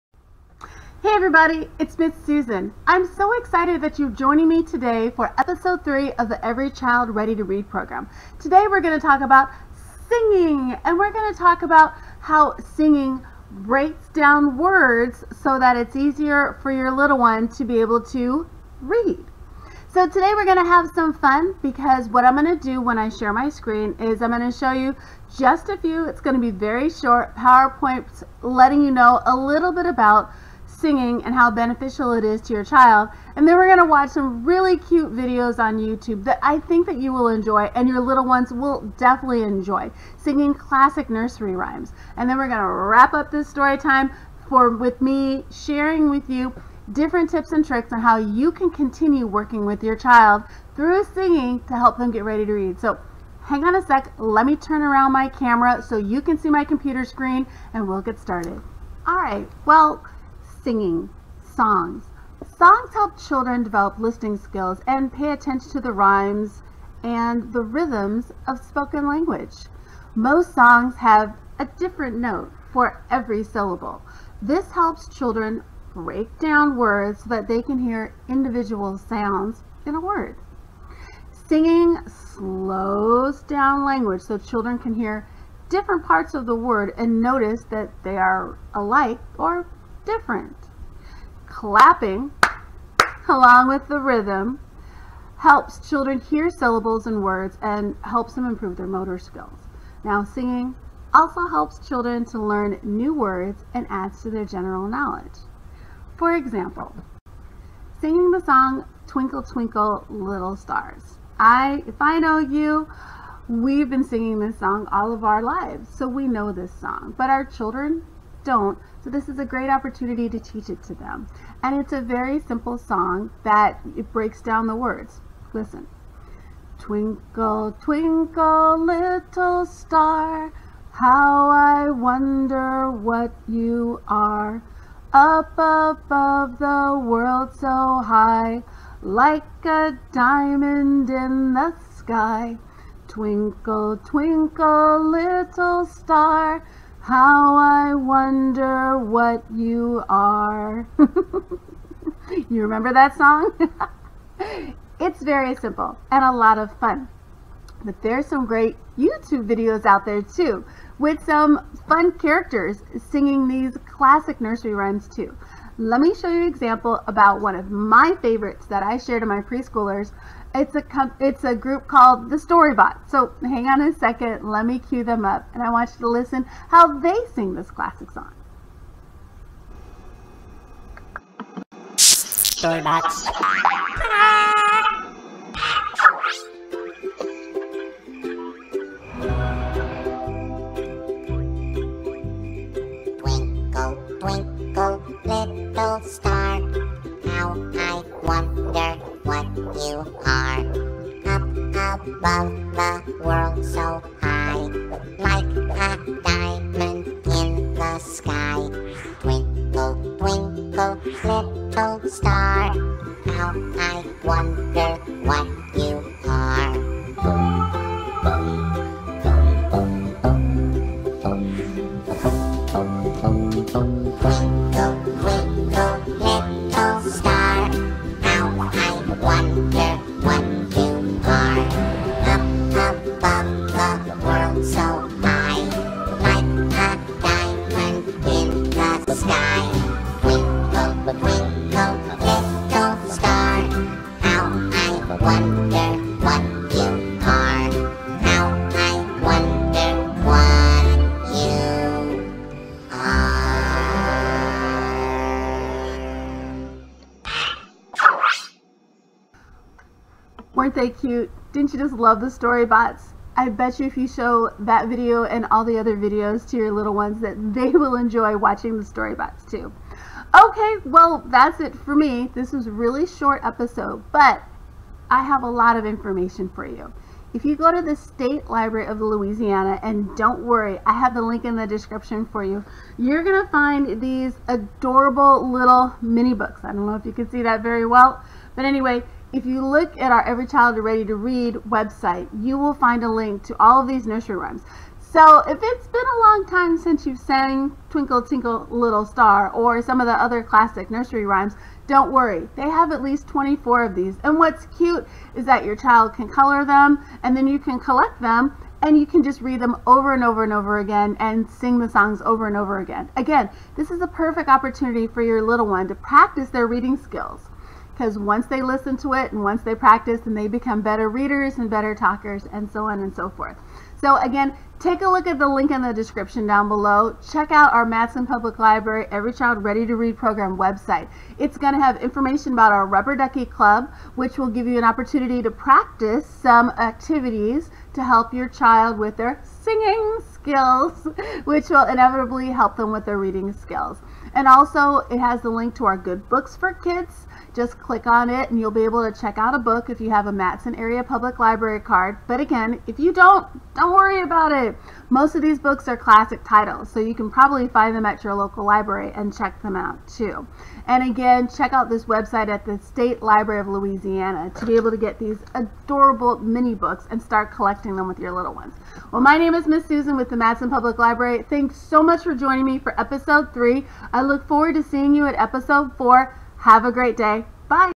ECRR - Ep. 3 Singing
In this episode, Ms. Susan from the Matteson Area Public Library District will show you how singing can help your child get ready to read.

If you would like to learn more about how to find a good book for your little one, click the

If you want to participate in the “Rubber Ducky Club,” click on this

Special Thanks to the Association for Library Service to Children and the Public Library Association who created the “Every Child Ready to Read” program and who has allowed me to create these videos for you.